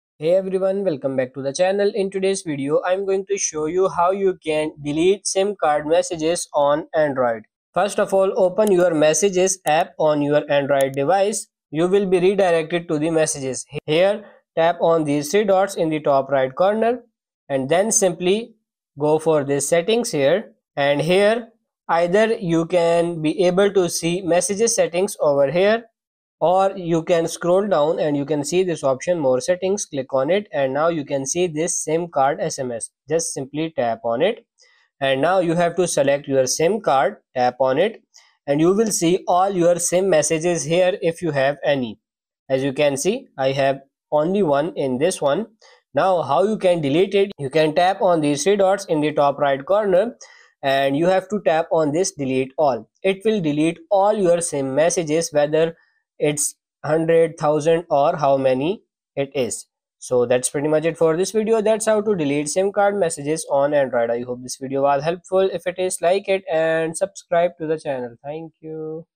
How to DELETE Sim Card Messages on Android
In today's video i'm going to show you how to delete sim card messages on android, easy and simple tutorial to follow.